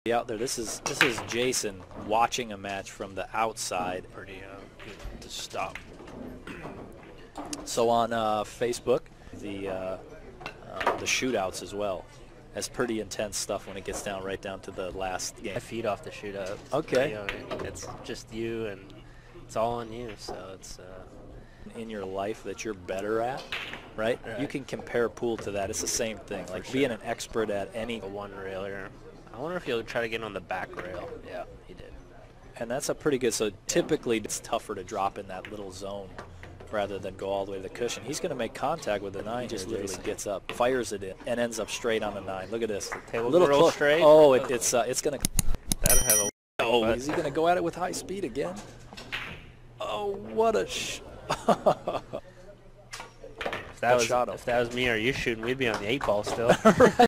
Bad Position Make You Miss | Jayson SHAW vs Joshua FILLER
World Pool Series - 2018 ARAMITH OPEN | 9 Ball
CREDIT: Billiard Network